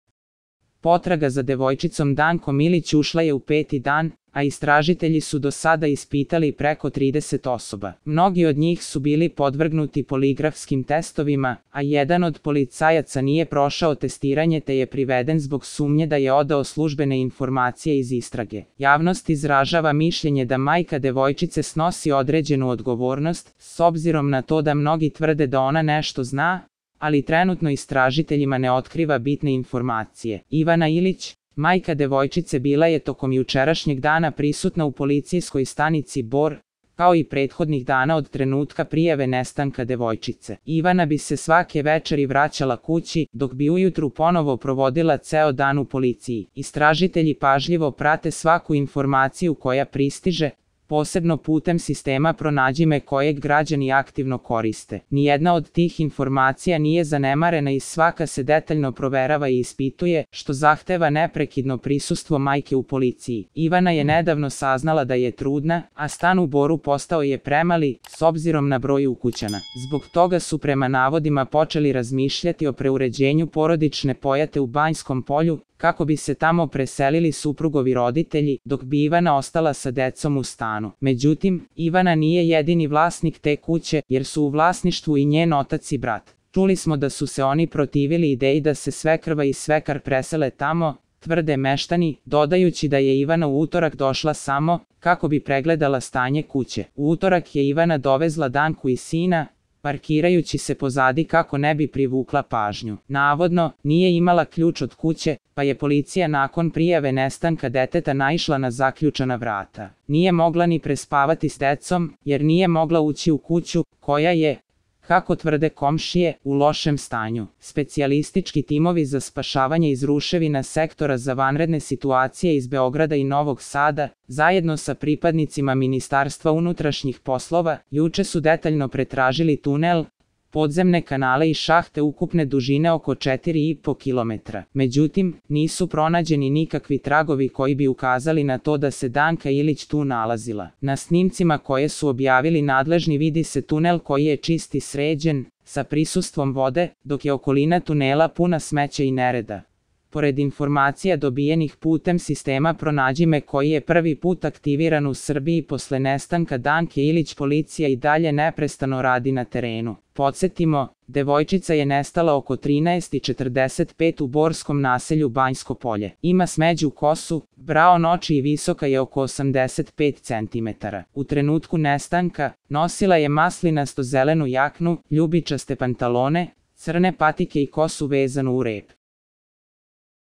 SLUČAJ DANKA ILIĆ - IVANA U POLICIJI, OTAC SE OGLASIO - PETI DAN POTRAGE
Ivana Ilić, majka male devojčice, provela je jučerašnji dan u pratnji pripadnika Ministarstva unutrašnjih poslova u policijskoj stanici Bor, kao i prethodnih dana od trenutka kada je prijavljen nestanak njene ćerke.

Nakon povratka kući samo na kratko predveče, majka Danke Ilić ponovo se rano jutros vratila u policijsku stanicu, gde je ostala tokom čitavog dana. Istražitelji pažljivo obrađuju svaku dobijenu informaciju, budući da neprekidno pristižu novi podaci, posebno putem sistema "Pronađi me", koji građani aktivno koriste.

Danka Ilić nestala je oko 13:45 u naselju Banjsko Polje u Boru. Karakteristična je po smeđoj kosi, braon očima i visini od oko 85 centimetara. U trenutku nestanka, devojčica je nosila maslinasto zelenu jaknu, ljubičaste pantalone, crne patike i kosu vezanu u rep.